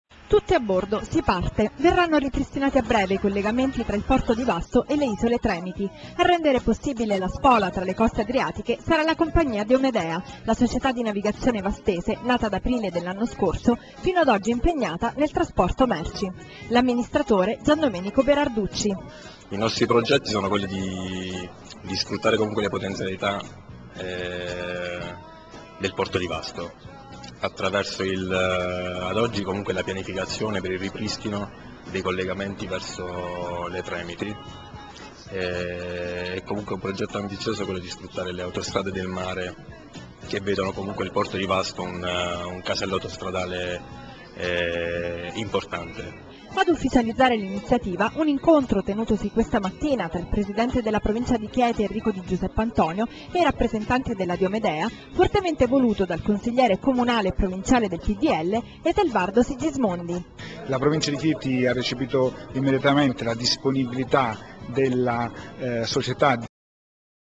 Da Vasto alle Isole Tremiti nel 2012
Lanciano, servizio TGMAX del 5 agosto 2011. Turismo: con un catamarano da 250 passeggeri sarà ripristinato nel 2012 il collegamento dal porto di Vasto alle Isole Tremiti. Due corse al giorno di andata e ritorno, nel periodo fra metà giugno e metà settembre.